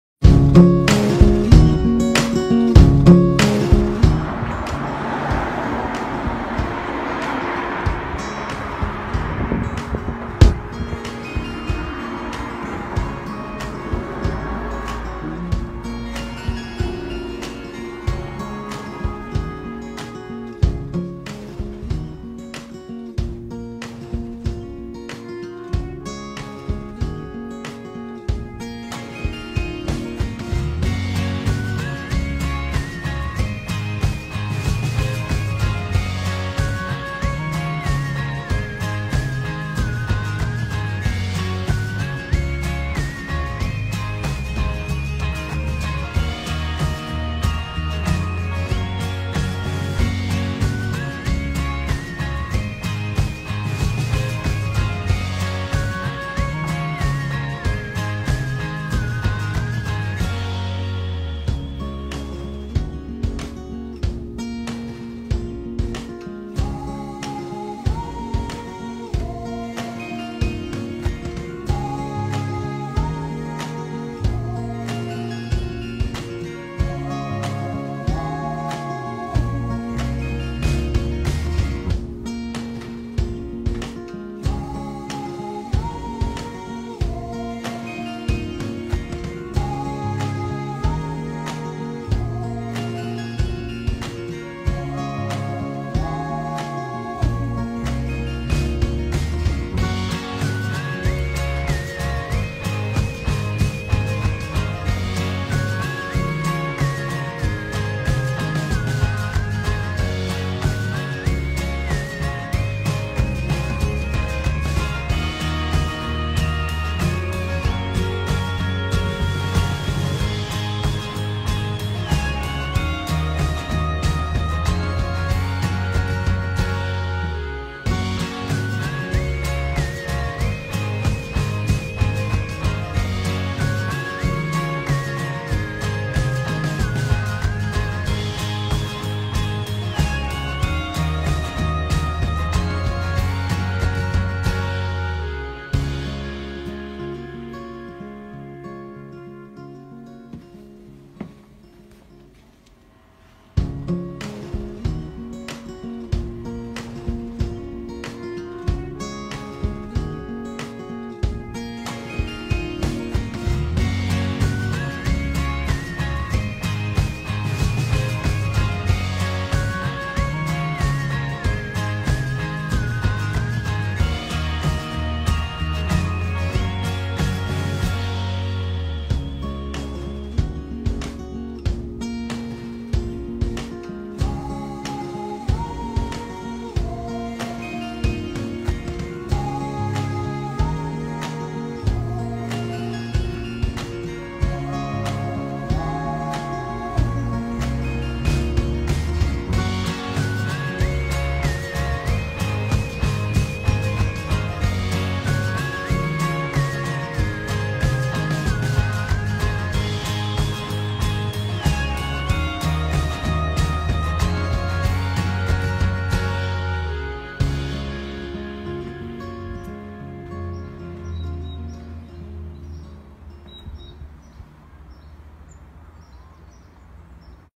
56 Lower Fold - Video Tour
A quick video tour around the beautiful, Victorian semi-detached property 56 Lower Fold in the heart of Marple Bridge